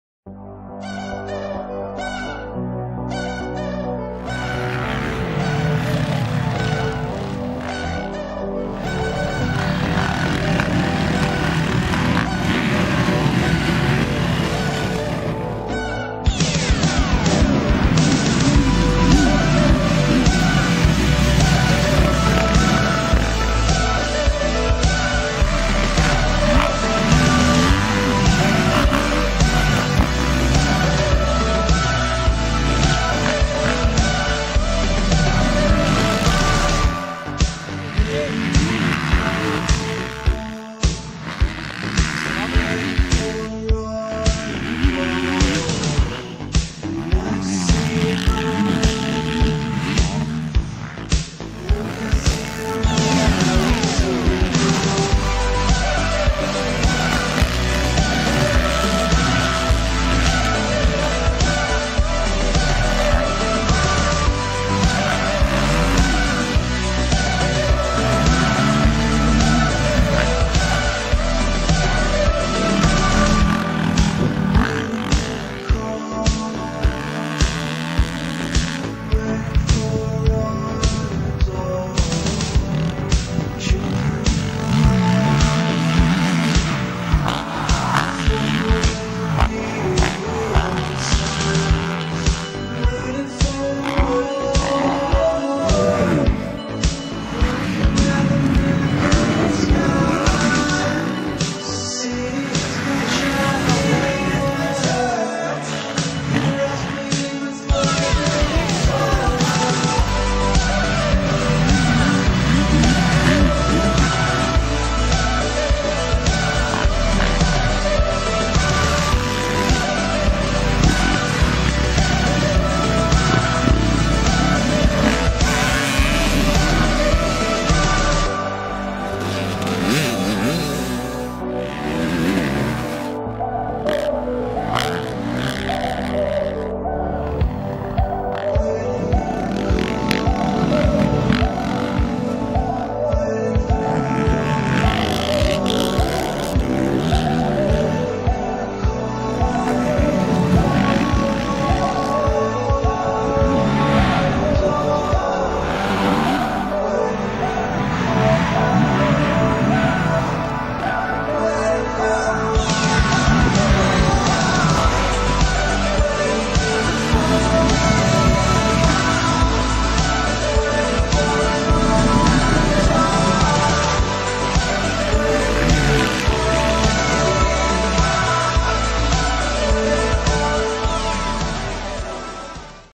Manuka mx park 2012 preview
quick video edit from Manuka mx park in Wanganui,NZ